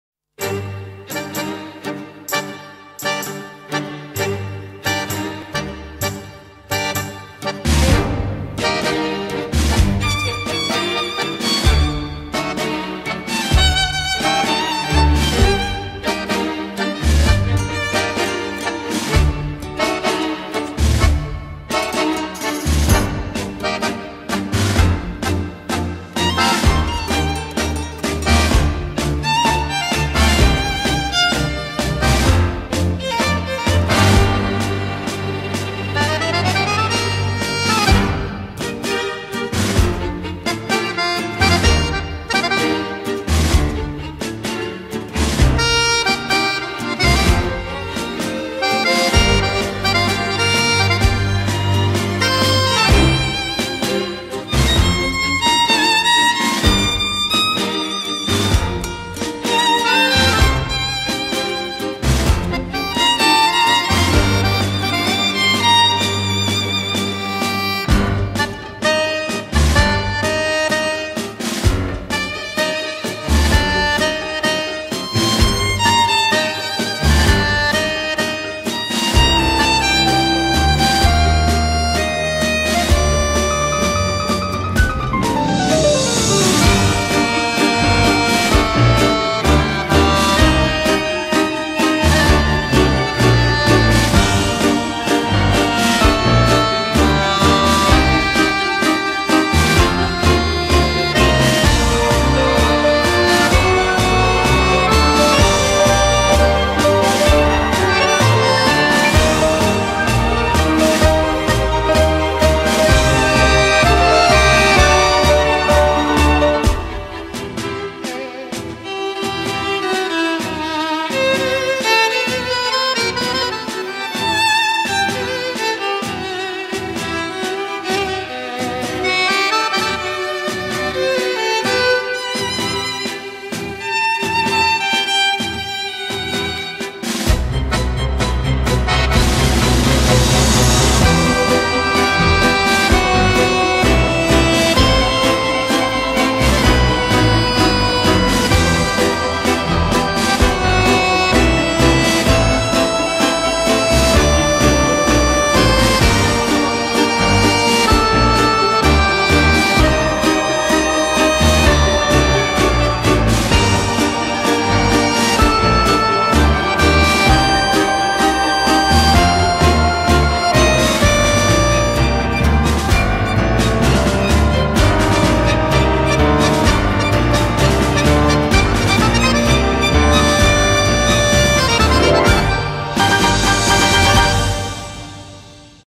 Танго! Подарок Maro karma! Swetlanaroma@mailru
Снова танго звучит и смеется, и плачет,
Снова вздрогнули нервы звенящей струной,
Снова время сметая, ты страстью охвачен...
Мы одни во Вселенной танцуем с тобой...

Наша тайна, которую знают лишь двое,
Сумасшедший наш танец, один на двоих...
То любя, то страдая, то в мире, то в ссоре
Мы танцуем, друг в друге сердца растворив...